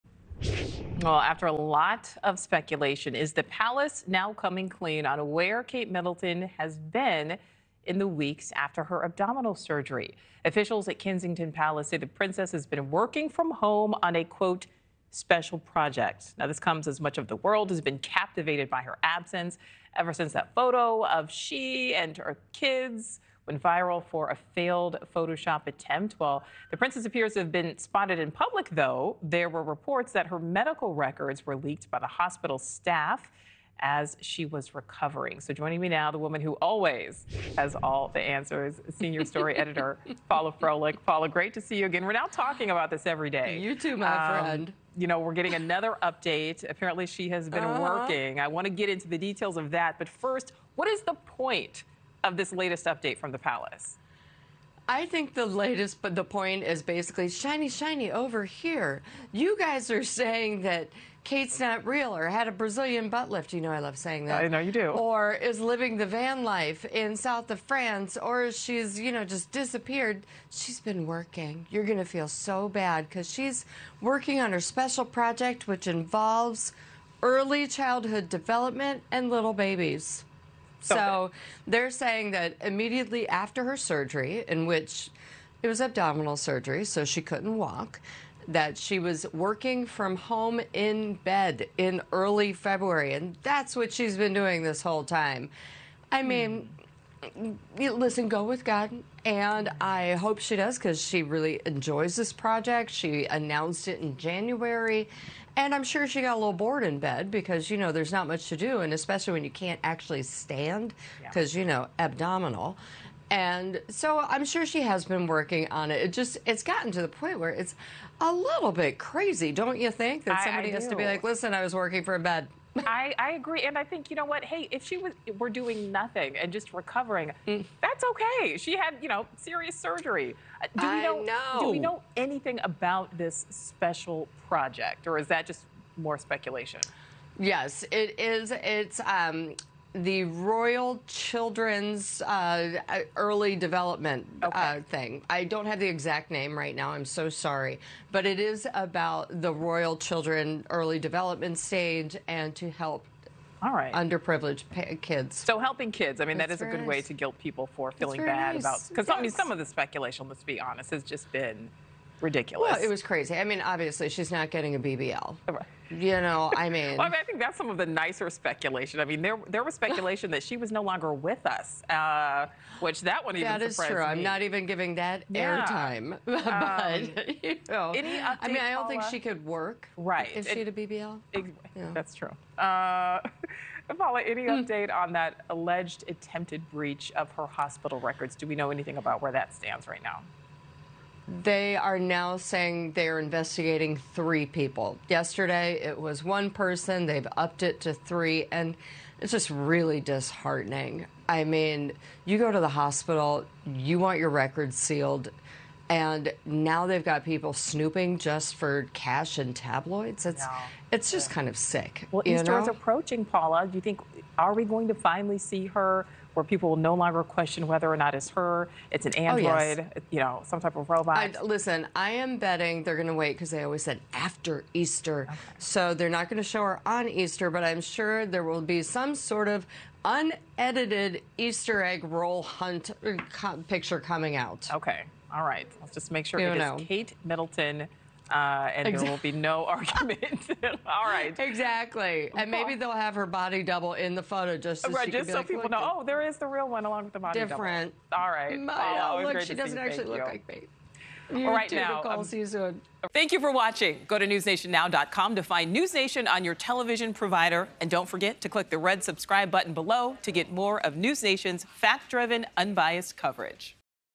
Princess Kate spotted in public after speculation on whereabouts | NewsNation Now
Questions continued about Princess Kate Middleton's whereabouts following her abdominal surgery weeks ago and whether she was OK. More speculation brewed after she posted a photo that she later admitted was altered. NewsNation senior story editor Paula Froelich joined "NewsNation Now" with the latest details on the mystery surrounding the royal.